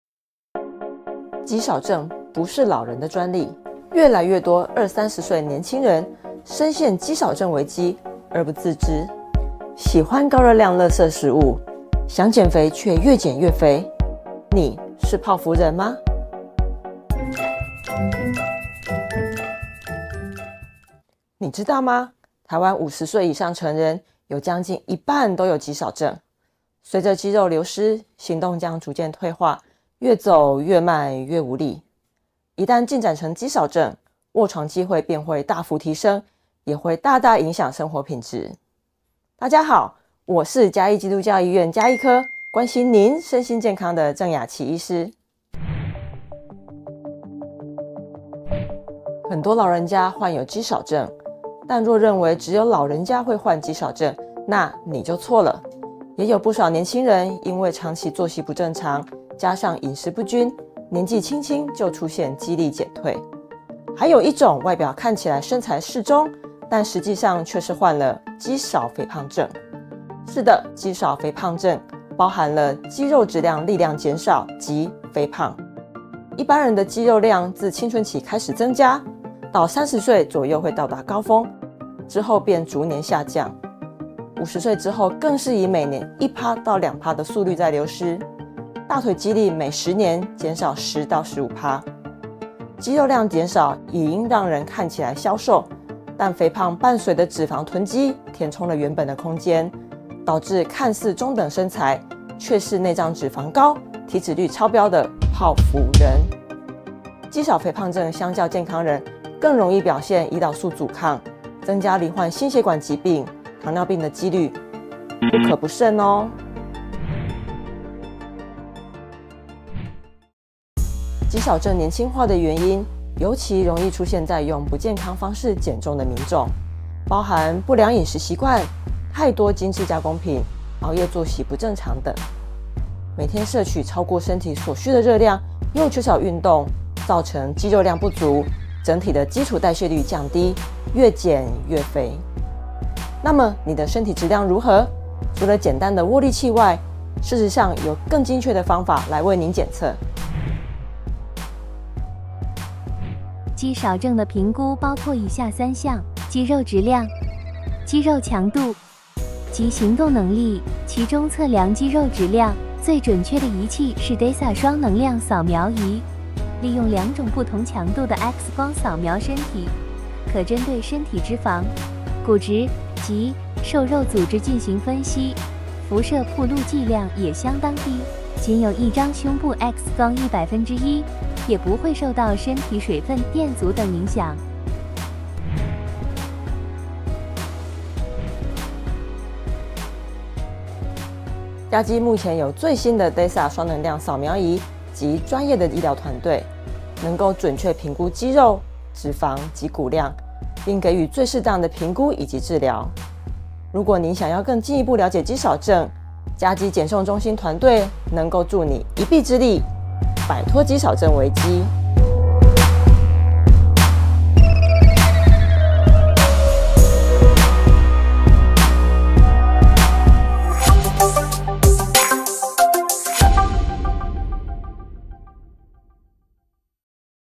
三分鐘認識肌少肥胖症 by 鄭雅綺 醫師
00:00  肌少症不是老人的專利
01:11  什麼是肌少肥胖症
01:58  泡芙人的成因
02:31  檢測身體肌力質量方法